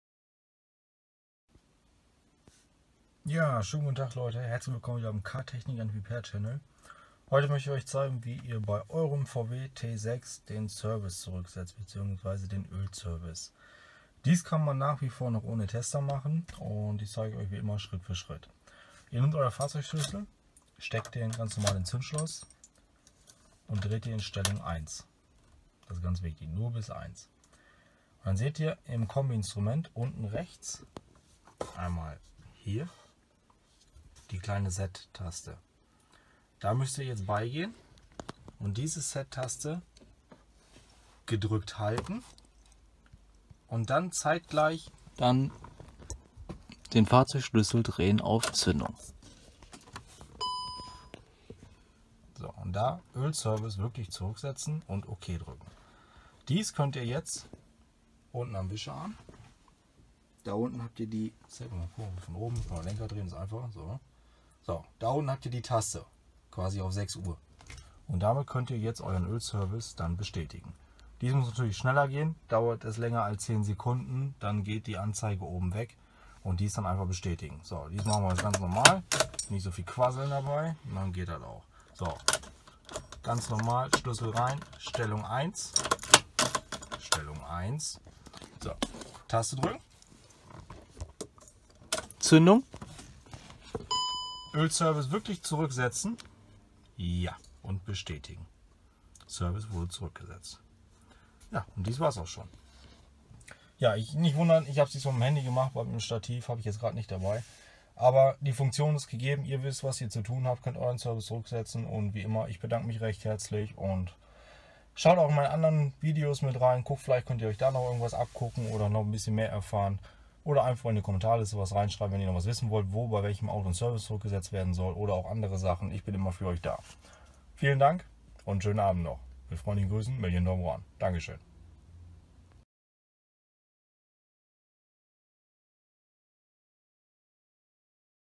VW T6 service zurückstellen - manuell ohne Tester
In diesem Video, zeige ich euch wie Ihr bei eurem VW Bus T6 den Service zurückstellen könnt. ..